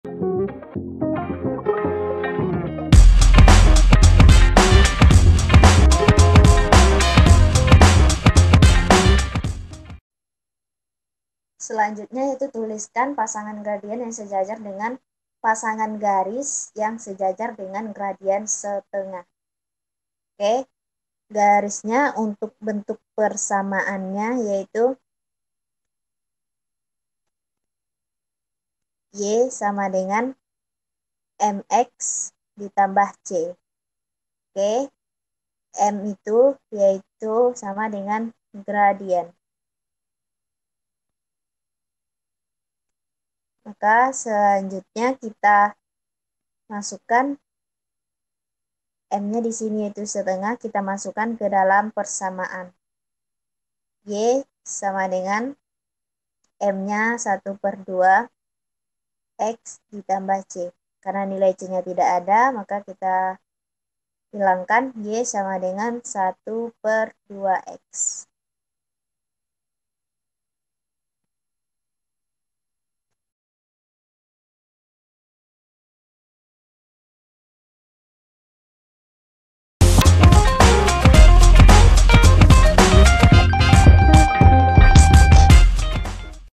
Tuliskan pasangan garis yang sejajar denga... | Matematika 2 Un | Kelas 8 | 3
✅ Dalam video ini kita akan membahas: Tuliskan pasangan garis yang sejajar dengan gardien berikut.
 \text{b}.{\frac{1}{2}}
Tema:
Kelas: 8
Mata Pelajaran: Matematika
Bab: 3
Buku: Matematika 2 Untuk SMP/MTS Kelas VIII

👉 Buku yang telah tersedia:
{PLAYLIST_LINK}
Di mana kamu dapat menemukan kami?